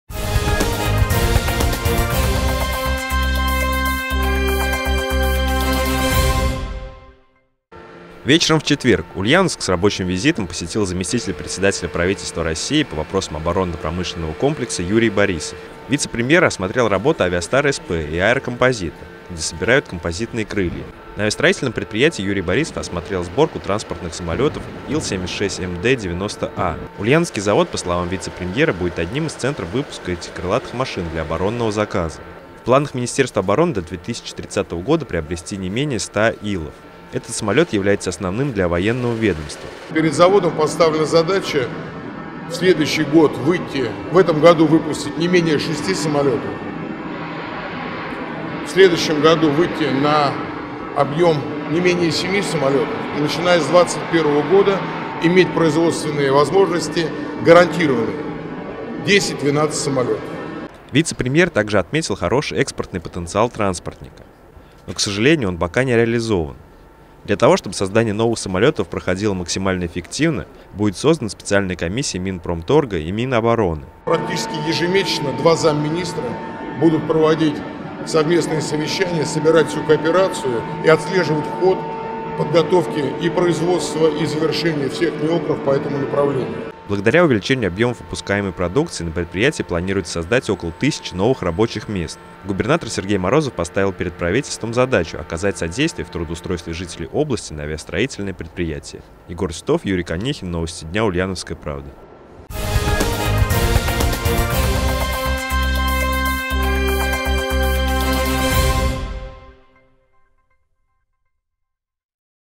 Министерство обороны закажет «Авиастару» более 100 Ил-76МД-90А
Вечером в четверг Ульяновск с рабочим визитом посетил заместитель председателя правительства России по вопросам оборонно-промышленного комплекса Юрий Борисов.